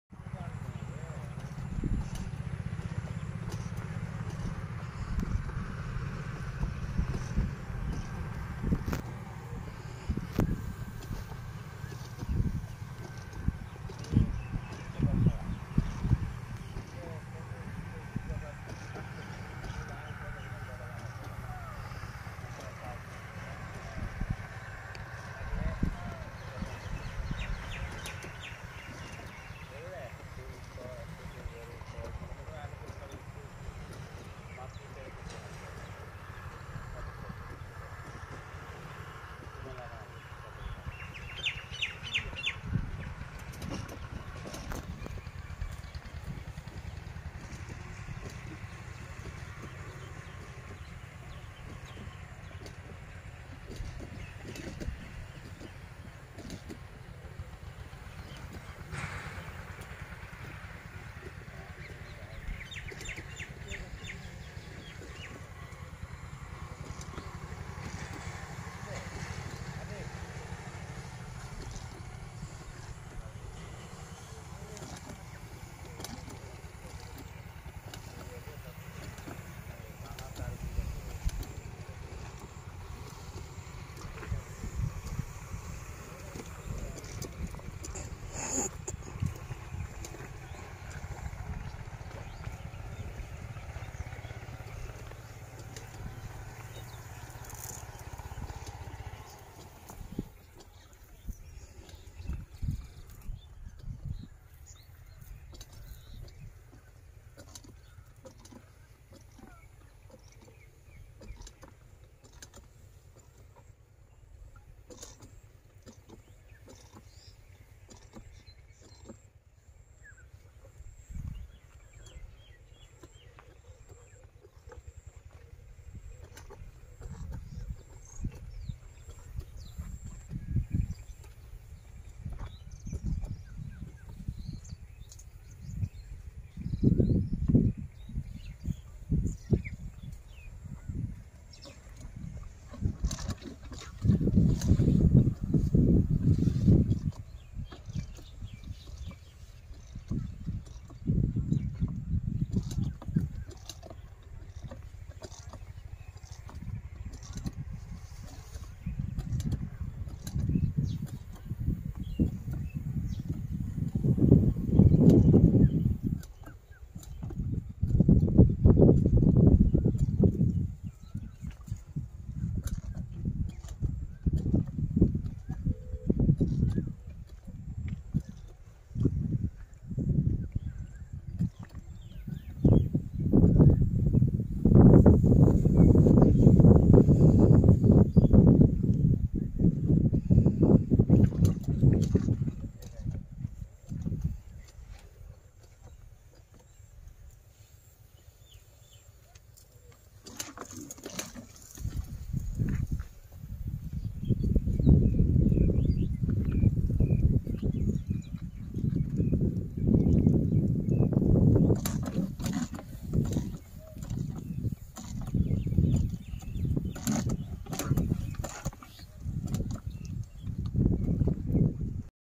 Dragon Fruit Cultivation// Dragon flowers in the local field//Dragon plants. Part-09
It is scenario of dragon fruit cultivation in a field of Bangladeshi local village area. They are self employed by cultivating the dragon fruit as a profitable business.